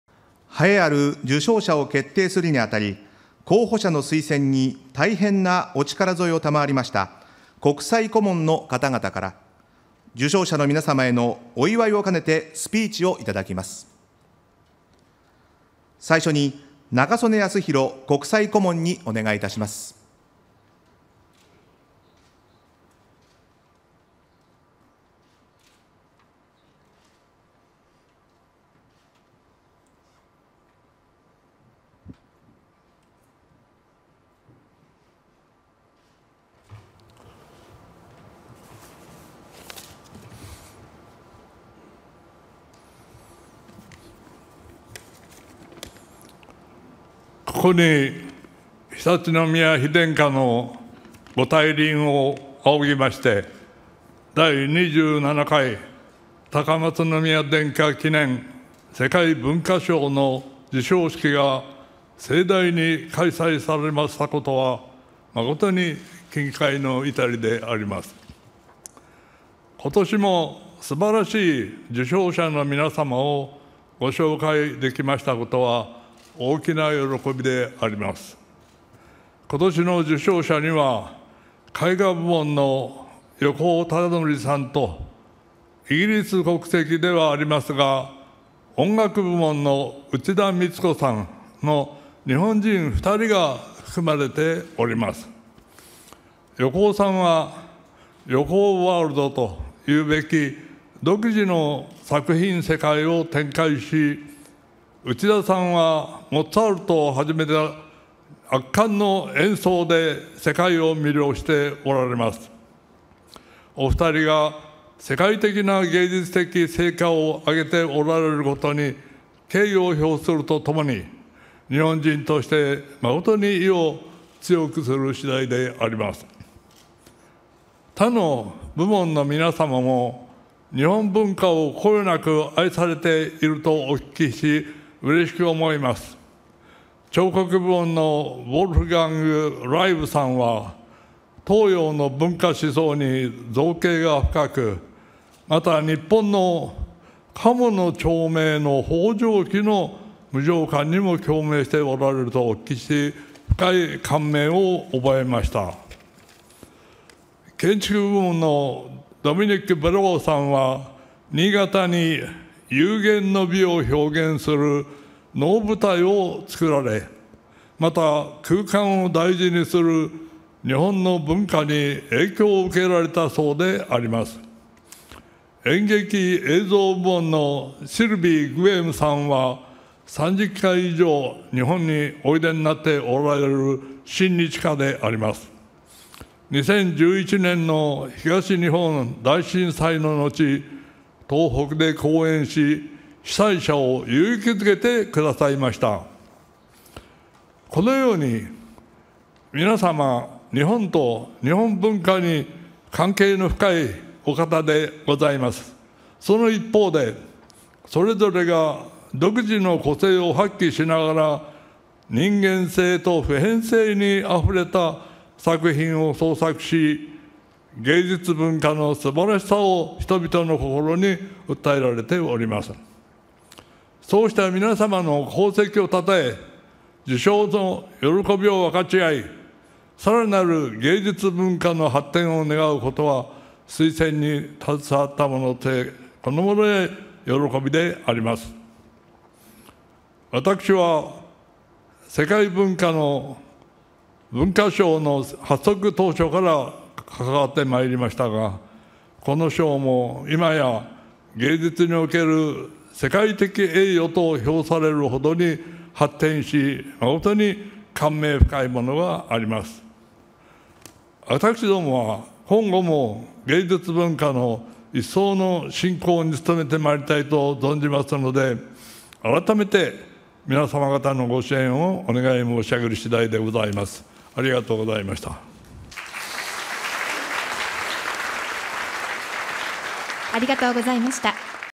【Official Video】A congratulatory speech by Mr. Yasuhiro Nakasone at the Awards Ceremony
A congratulatry speech by Mr.Yasuhiro Nakasone, International Advisor to Praemium Imperiale, former Prime Minister of Japan.